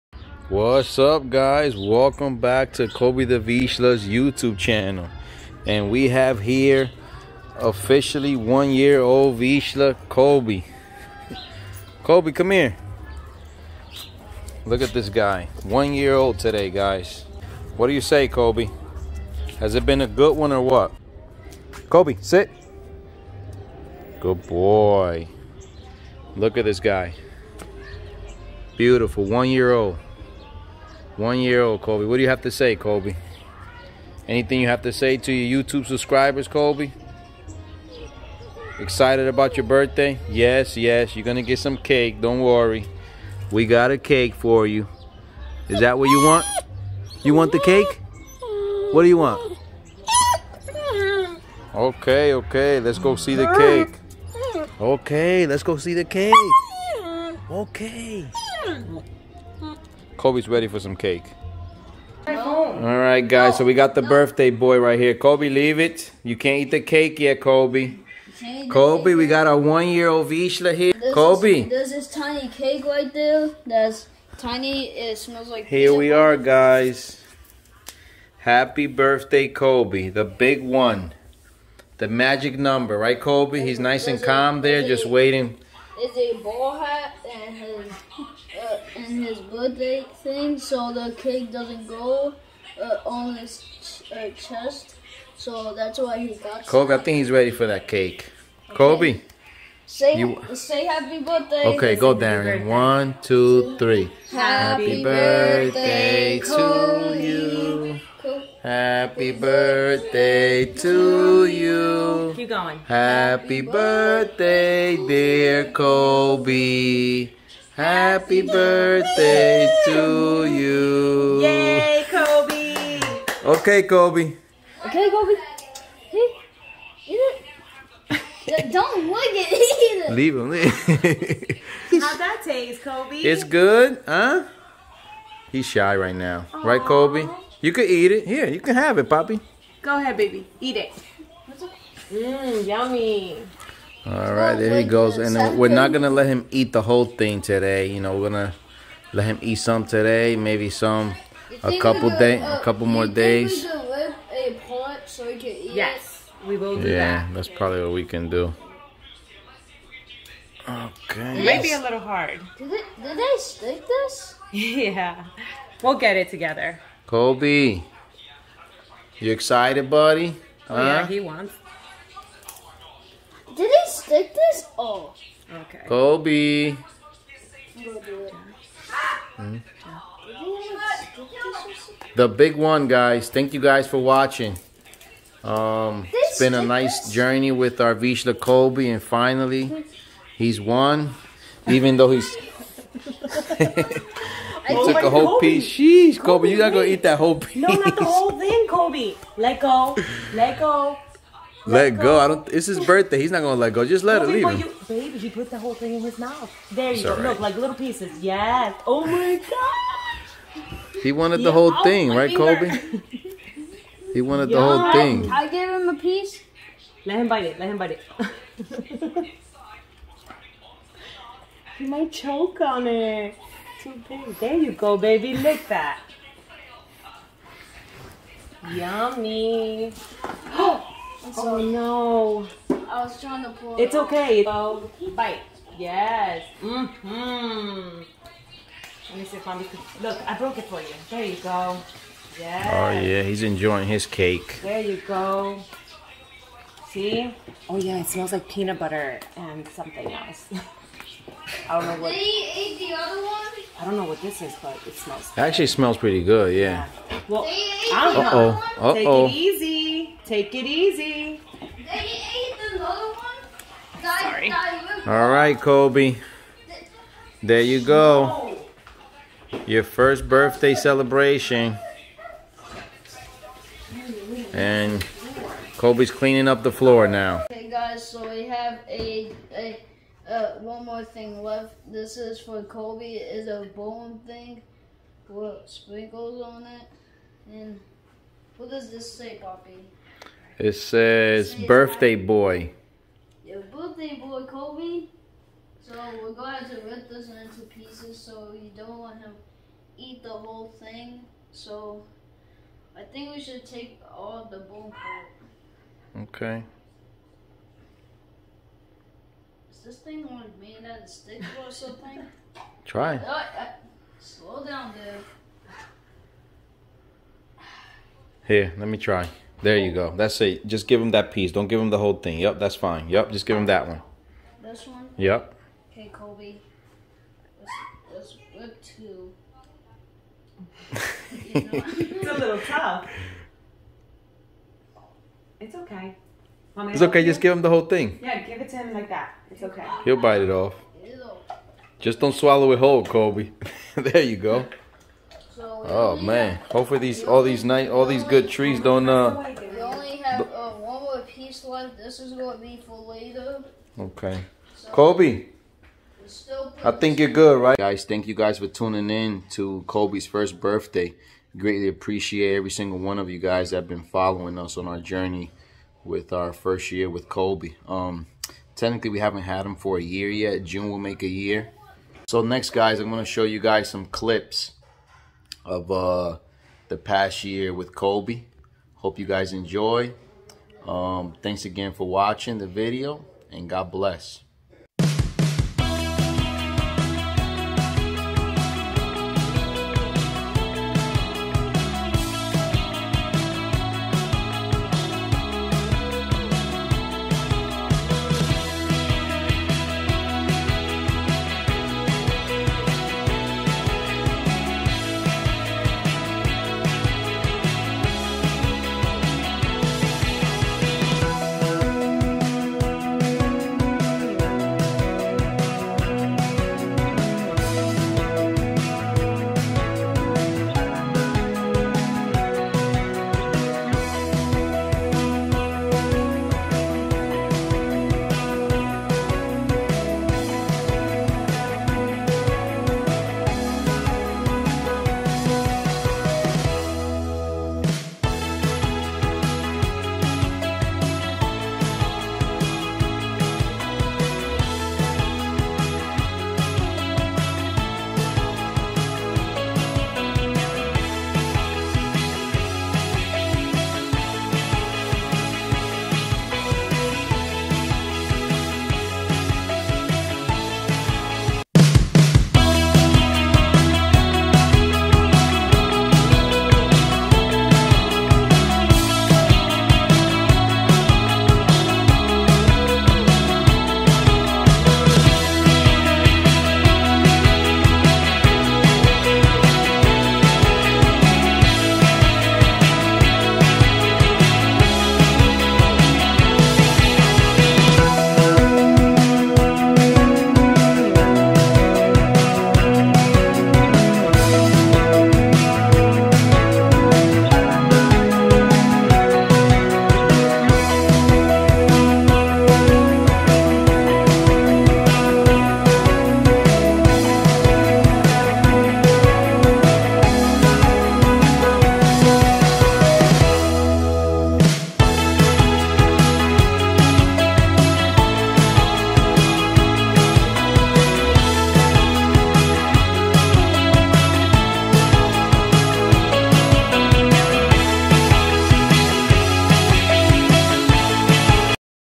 Kobe The Hungarian Vizsla First Birthday
Happy Birthday Kobe. Kobe The Hungarian vizsla turns 1. Birthday celebration. A look back at the past year with Kobe.